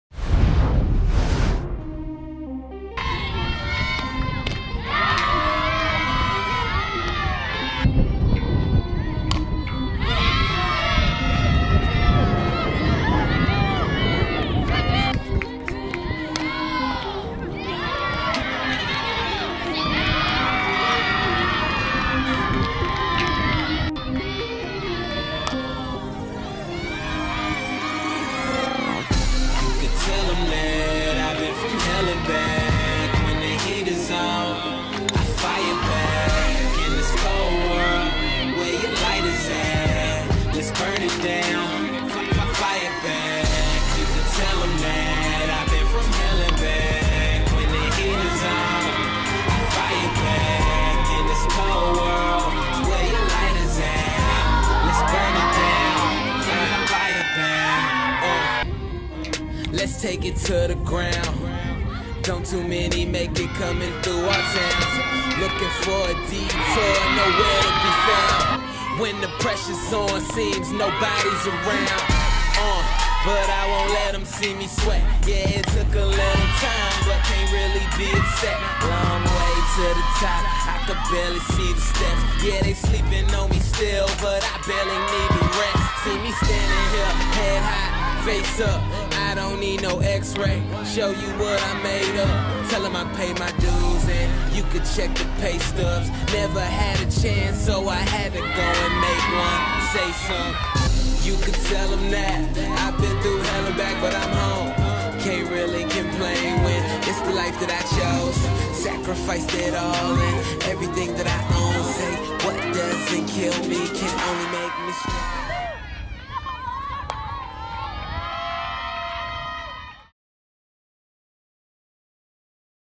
EIU Softball highlights vs SIUE (4/9/19)
The Panthers Softball team hosted SIUE for an OVC doubleheader on April 9th, 2019.